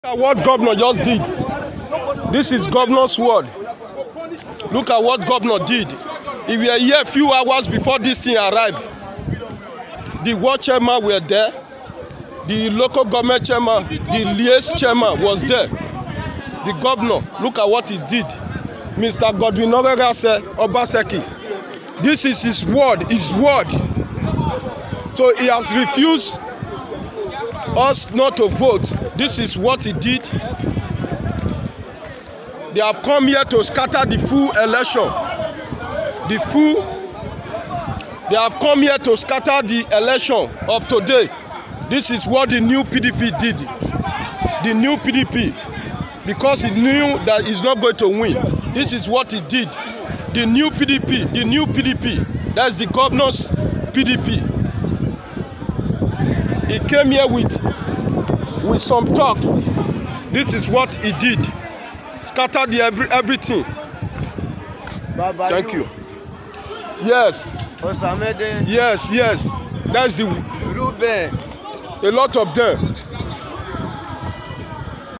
Accusations after violence broke out at the EDO State polling Unit during the 2023 election.
Accusations after violence broke out at the EDO State polling Unit during the 2023 presidential elections